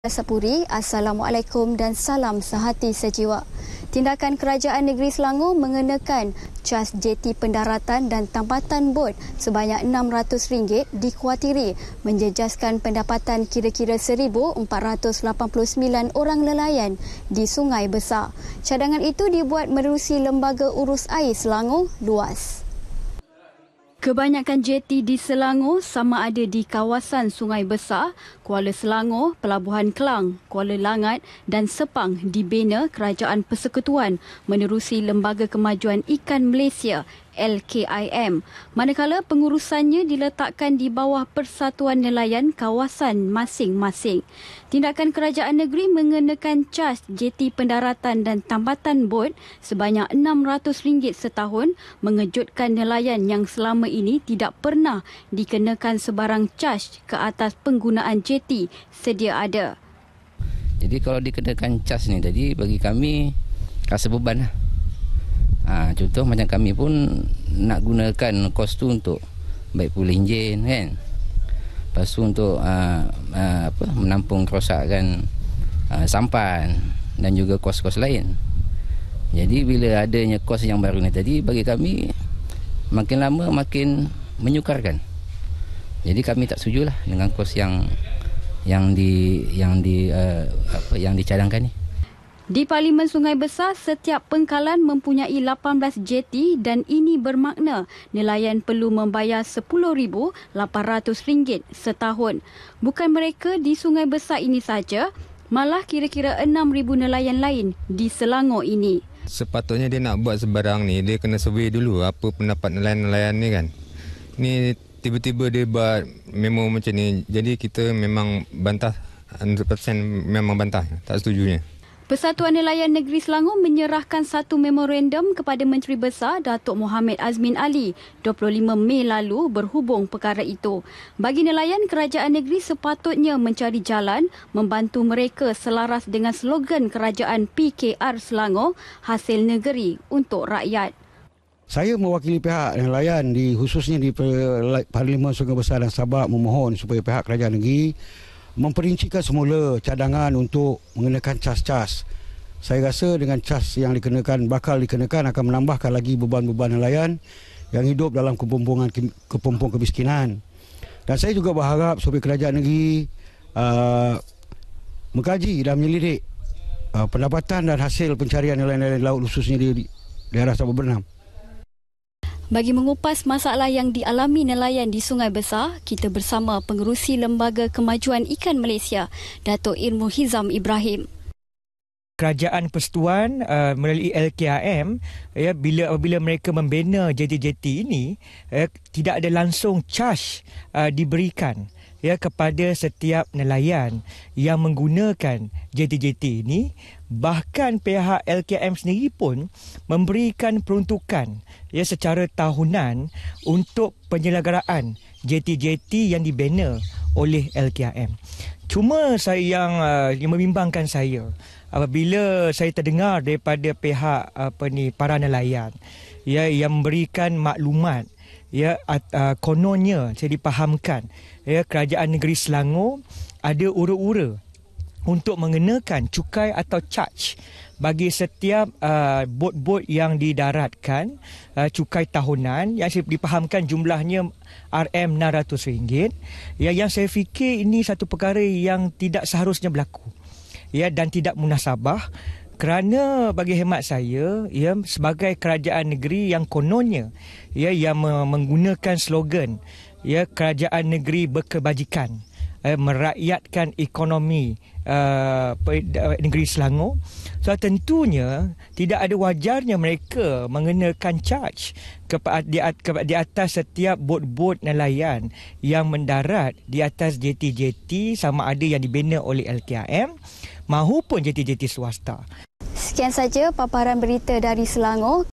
LANGSUNG DARI SELANGOR - 5PM [1 JUN 2016]
PENYAMPAI : SAMSIA SAMSI
LOKASI :  JETI PENGKALAN NELAYAN,SUNGAI NIBONG

Fokus berita dari Selangor kali ini ialah masalah nelayan di Selangor yang dikenakan caj  pendaratan jeti dan tambatan bot, khususnya di Sungai Besar.